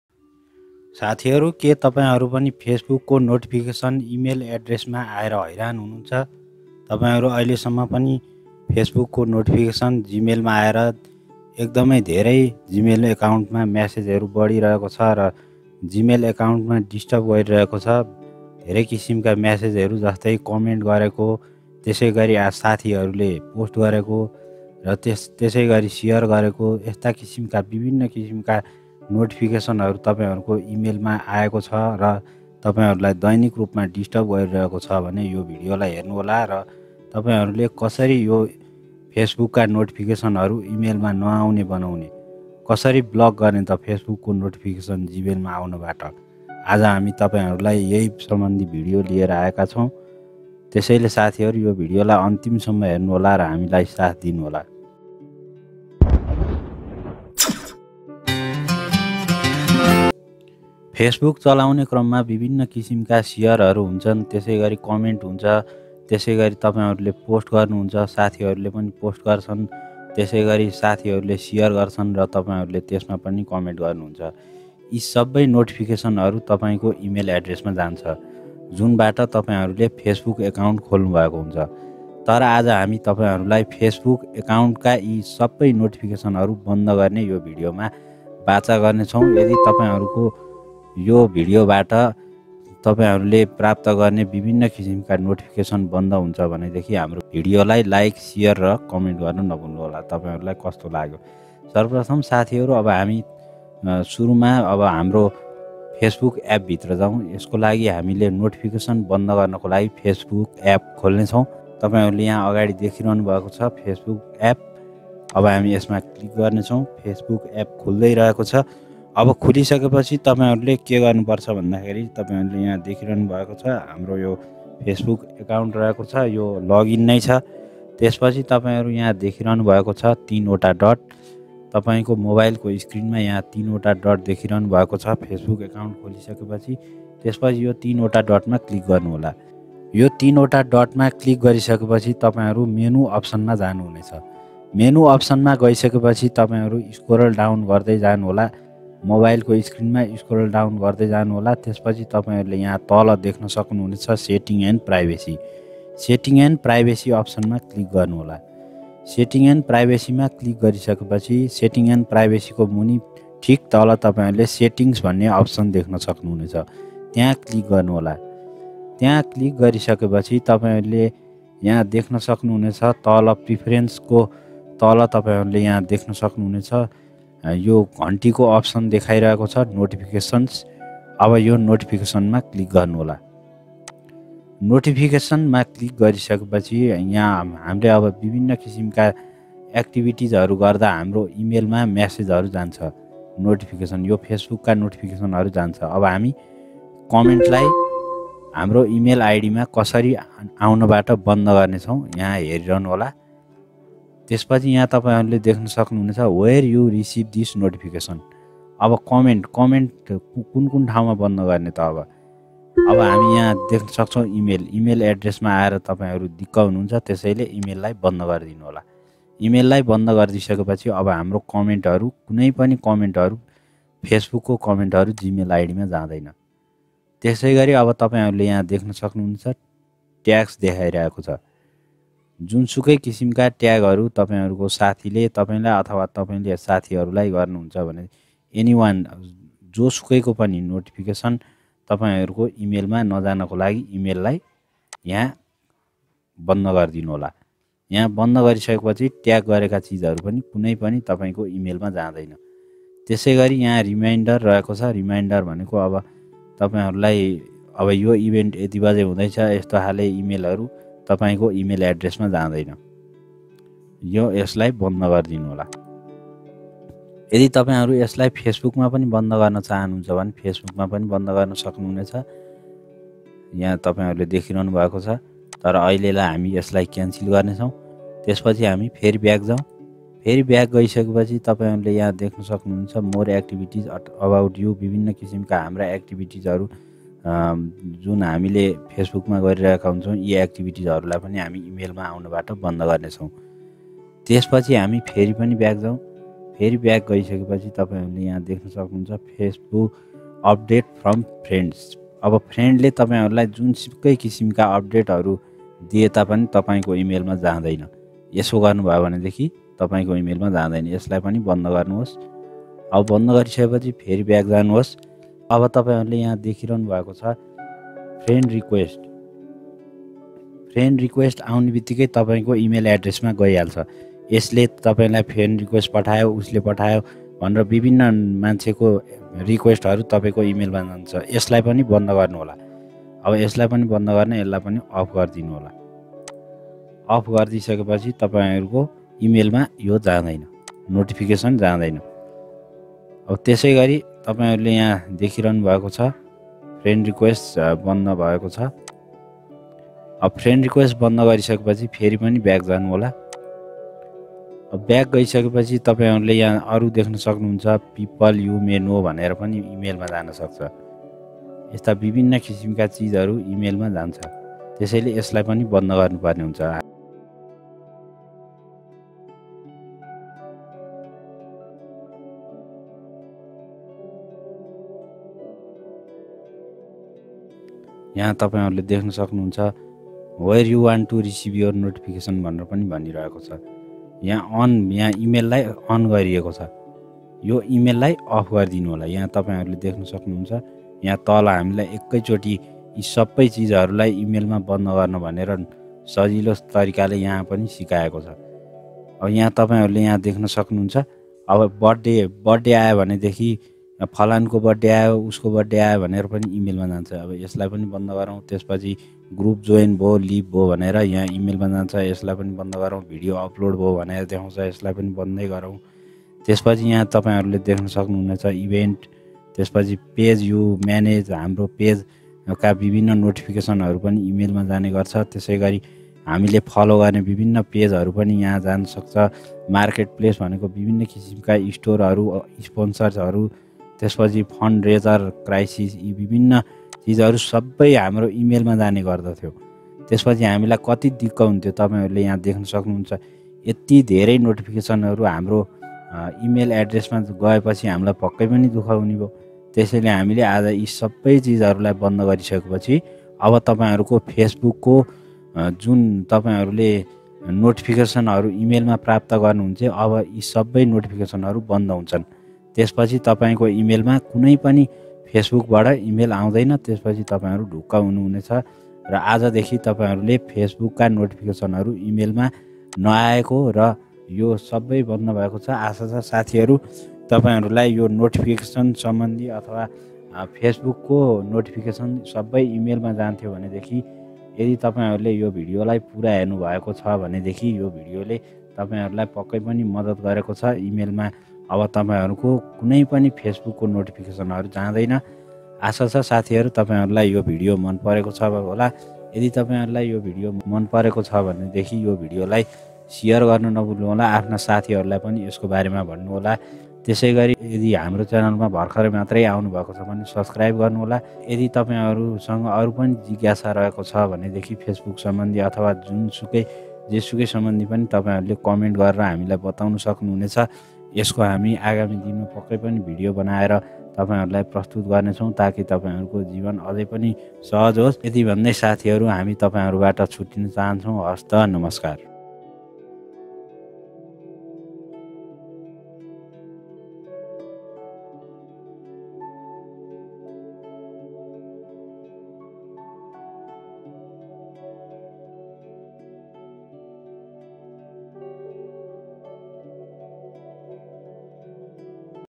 Stop Facebook Notification in Gmail Account | Block Facebook Notification to Gmail |Learn New Things
Welcome to Learn new things channel.

how to stop facebook notifications

Video Details:
- Video Introduction about Facebook Notification 00:00-00:55
- Learn New Things Channel Intro 00:55-01:00
- How to stop Facebook Notification to G-Mail Account 1:01 - 07:56
- Conclusion on the video topic to block facebook notification 07:57: 08:52
- Ending Intro of Video and another Topic Video 08:52-09:12